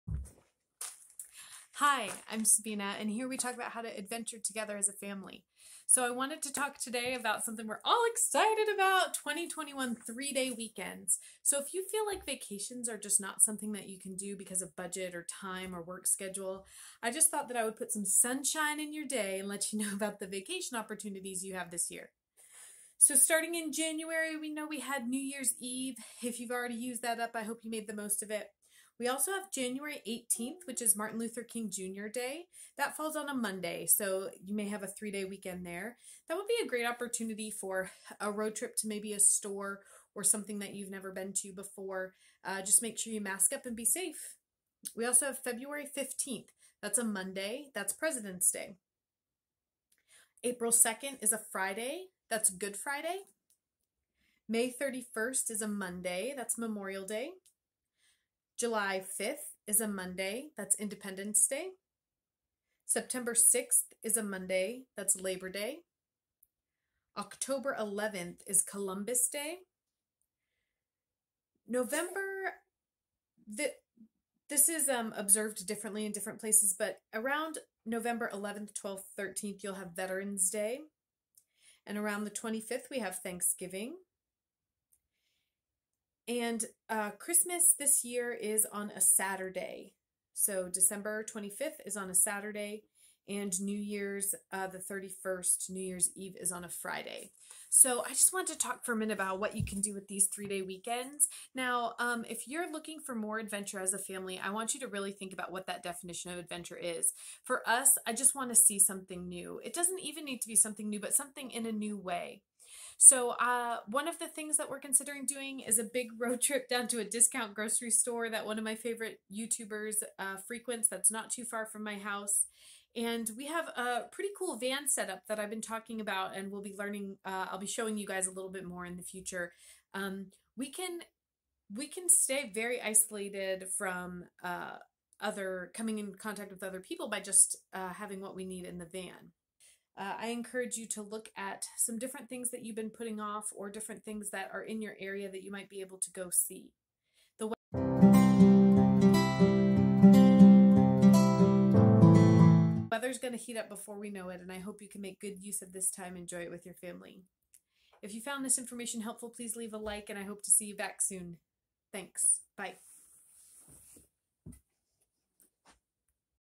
The BEST Time to Vacation This Year | 3 Day Weekends 2021
TWENTY TWENTY ONE! Here are the US observed Holidays that could score your family a 3 day weekend! I hope this information can help you get fired up about more and more family adventures.

Timestamps:
1:41 Start of Dates
0:22 Ideas and Inspiration

Favorite Gear for my Van:
Hot Logic